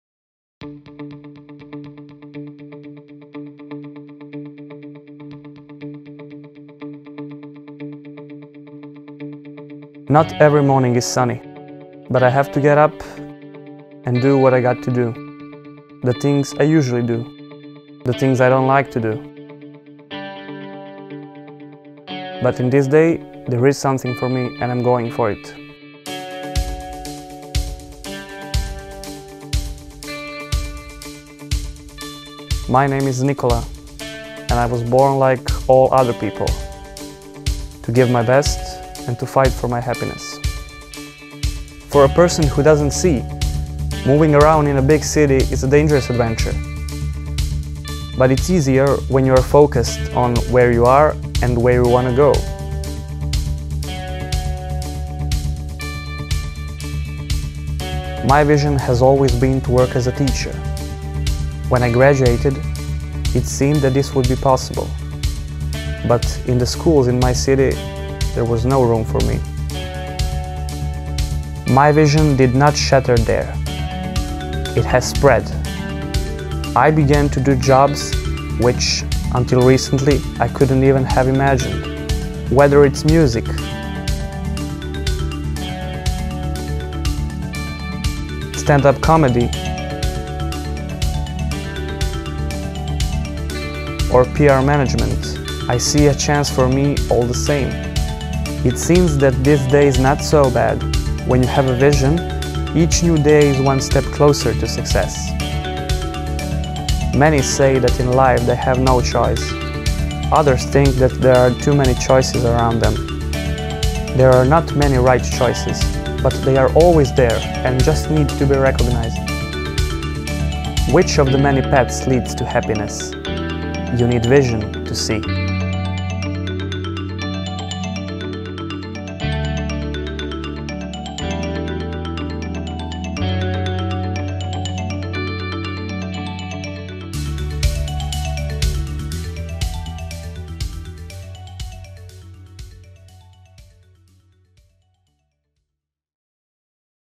Vision Final Conference 2016
Storyteller: Nikola Radojlović

Established in 1993, the ESTIEM project Vision is an Europe-wide seminar series with an annually changing, unconventional topic related to Industrial Engineering and Management. Materials - Key Drivers for Innovation" was selected as the focus topic for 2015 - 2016.

Local Group Belgrade has the honor to organize  Vision Final Conference
this year and make a summary of all the topics from previous 10  Vision events.
From 9th till 15th October 35 students from all around Europe will have a chance to come to Belgrade and take part in this project.
The Final Conference will consist of many interactive lectures, visiting companies, workshops and case studies.